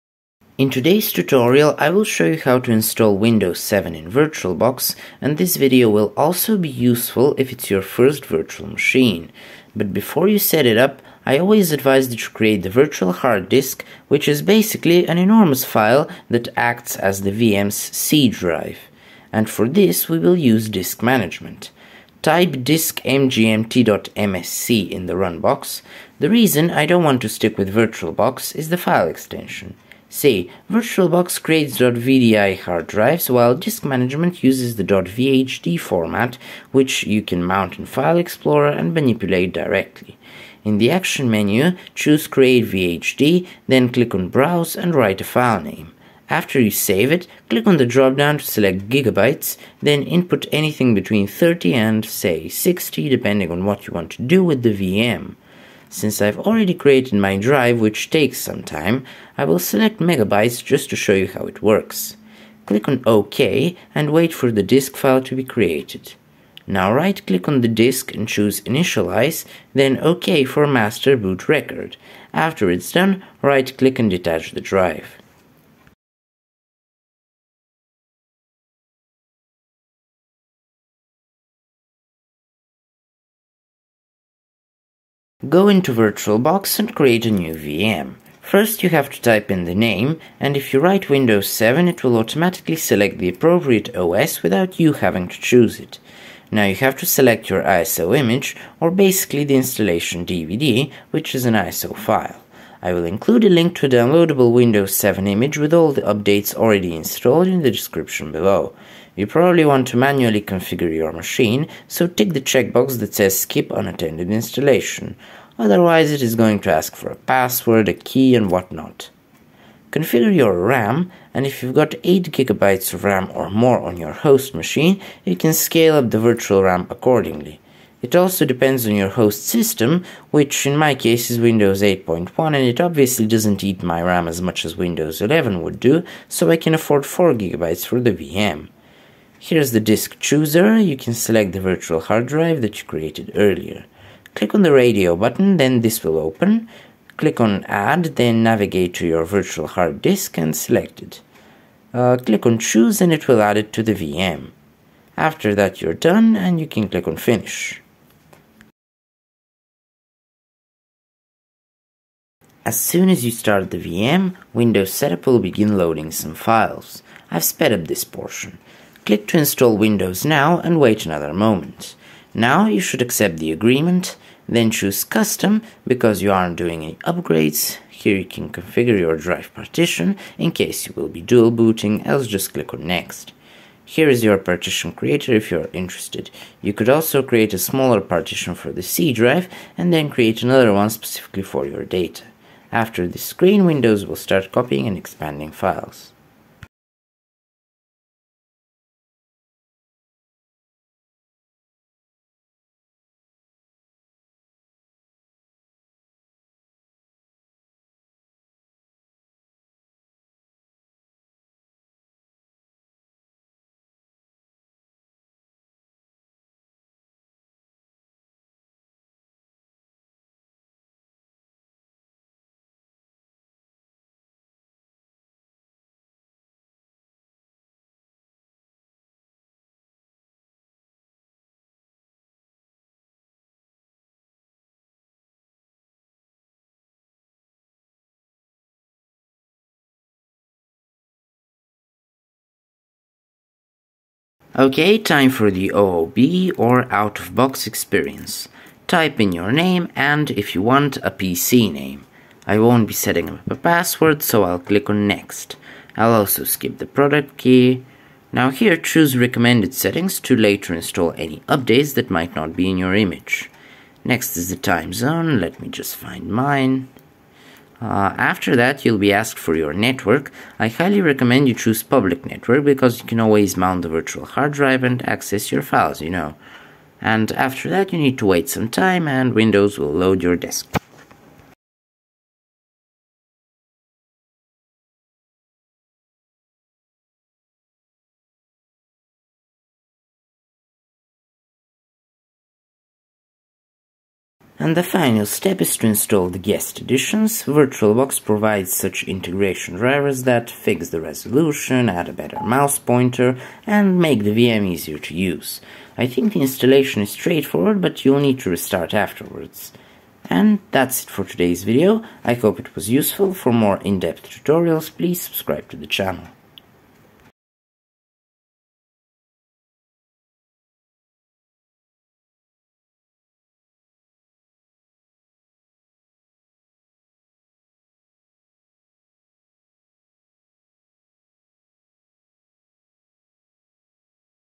How to install Windows 7 in VirtualBox
This video shows you how to install Windows 7 in VirtualBox, including the drivers and the updates, in 2024. Windows 7 is a rather old and mostly unsupported OS by now, but if you want to install it for the nostalgia, follow the steps in this video. This tutorial is also helpful if it is your first VirtualBox VM as I explain step-by-step for beginners in the field of virtualization. VirtualBox is one of the easiest and most streamlined hypervisors out there, and it's great for both hobbyists and advanced PC users.

Chapters:
00:00 - Creating the virtual hard disk
01:23 - Configuring the VM
02:40 - Setting up Windows 7
04:02 - OOBE
04:52 - Guest additions
05:10 - Thanks for watching! Please subscribe!